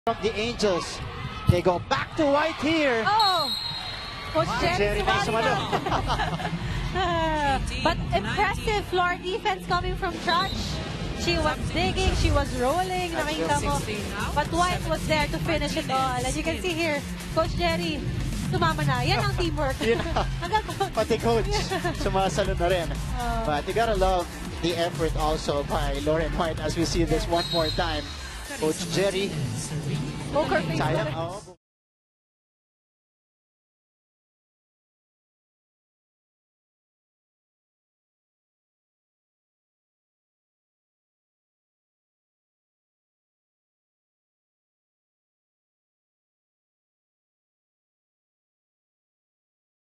PVL RC Season 2: Lauren Whyte with the down-the-line to... coach Jerry? | IRG | Best Plays
Coach Jerry Yee joins the fun, as he pulls out a textbook reception to Lauren Whyte's down-the line hit.